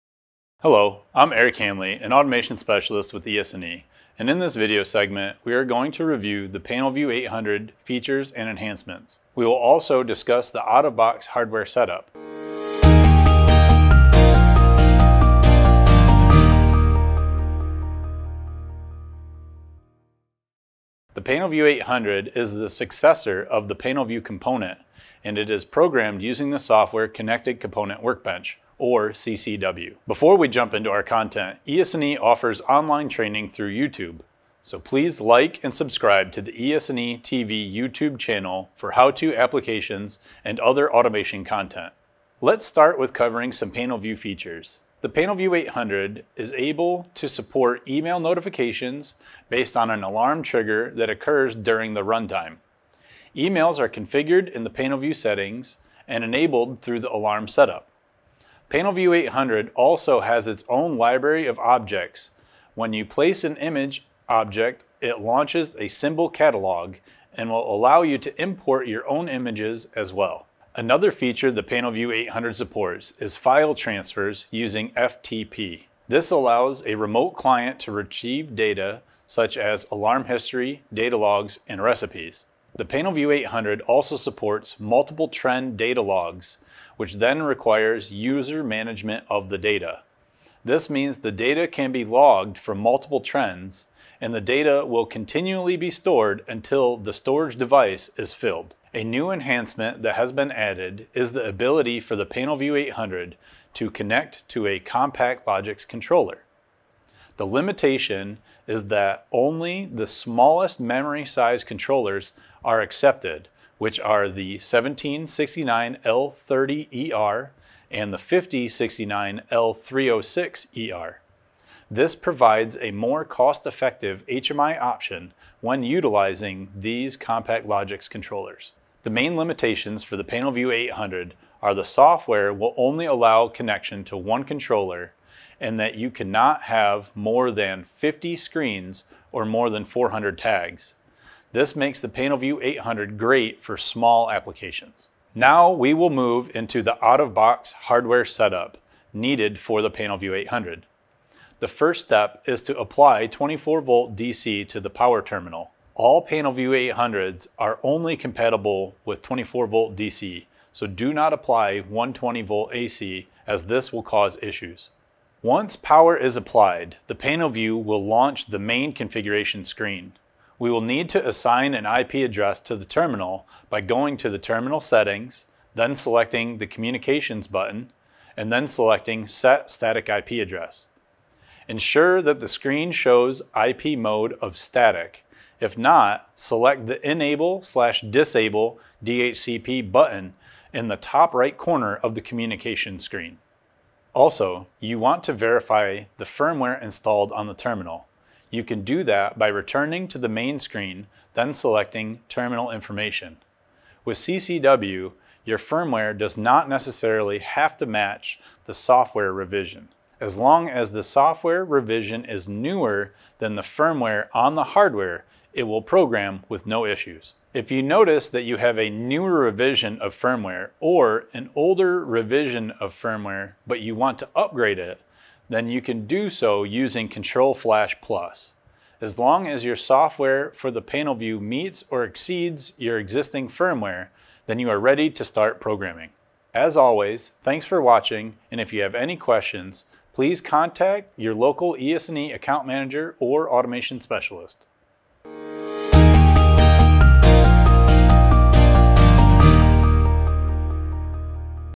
Features and Out Of Box Setup - Panelview 800 and CCW - Part 1
Eric Hanley, an Automation Specialist with ES&E, reviews the PanelView 800 features and enhancements including out-of-the-box hardware setup. The PV800 is the successor of the PanelView component and is programmed using the software-connected components workbench or CCW.